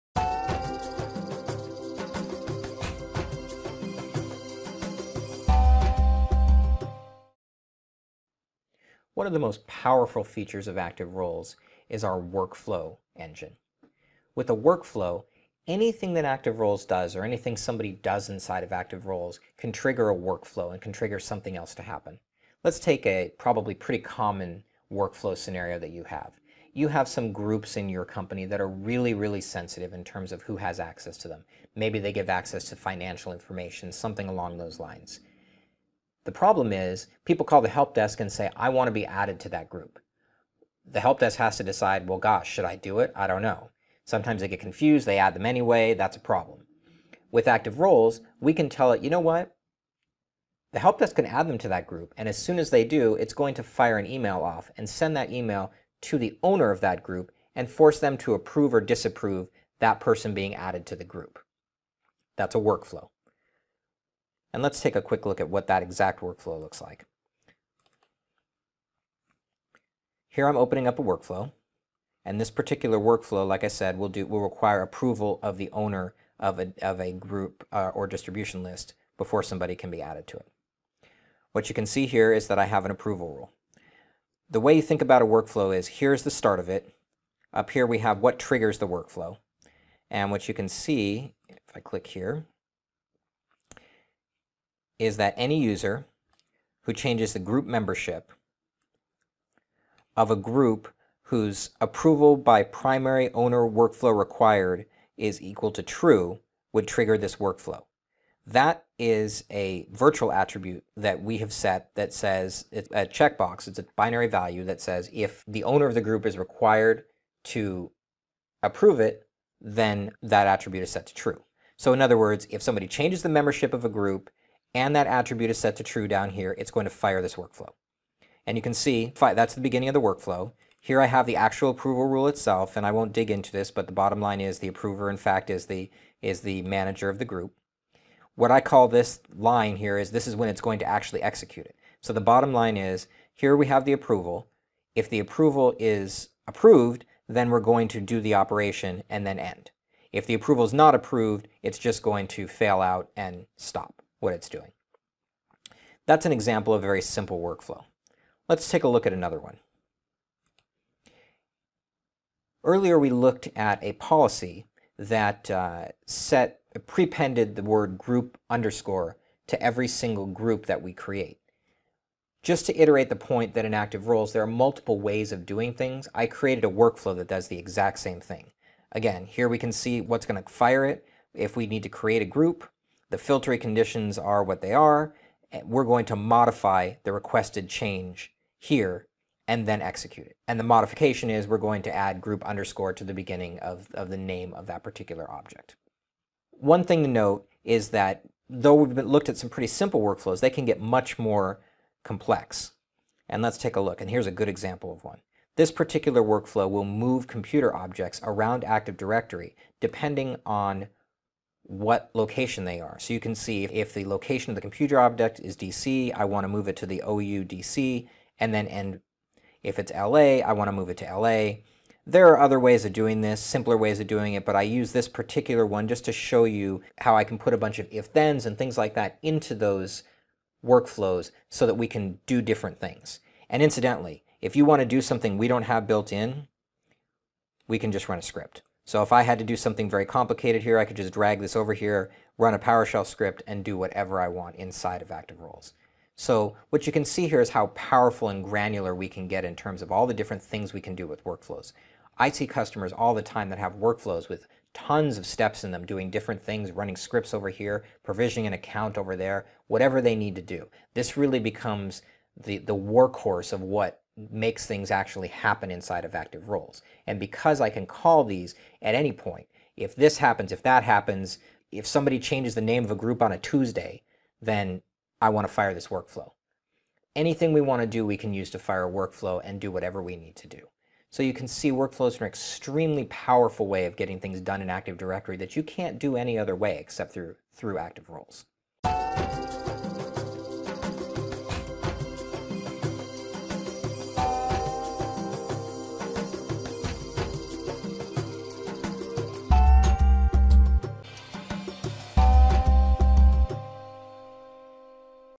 One Identity Active Roles | Overview #9 | Workflow
Training Video Series: One Identity Active Roles Overview
- Workflow

Active Roles provides a rich workflow system for directory data management automation and integration. Based on Microsoft’s Windows Workflow Foundation technology, this workflow system enables IT to define, automate and enforce management rules quickly and easily. Workflows extend the capabilities of Active Roles by delivering a framework that enables combining versatile management rules such as provisioning and de-provisioning of identity information in the directory, enforcement of policy rules on changes to identity data, routing data changes for approval, e-mail notifications of particular events and conditions, as well as the ability to implement custom actions using script technologies such as Microsoft Windows PowerShell or VBScript.